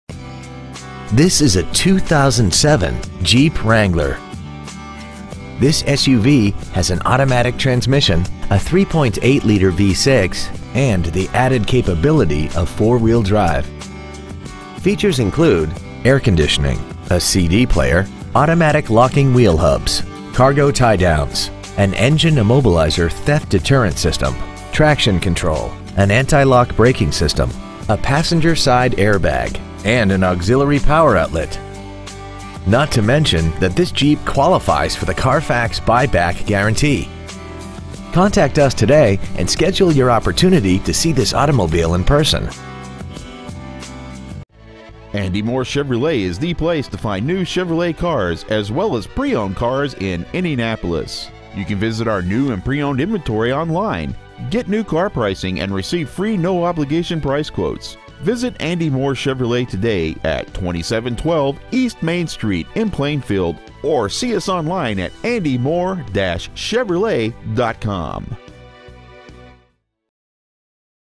2007 Jeep Wrangler Indianapolis IN 46168
Year : 2007
 Make : Jeep
 Model : Wrangler
 Engine : 3.8L V6 12V MPFI OHV
 Trans . : Automatic
 Exterior : Flame Red
 Miles : 94,694
 Interior : Dark / Medium Slate Gray
 Stock : PC3070A

 2712 E. Main Street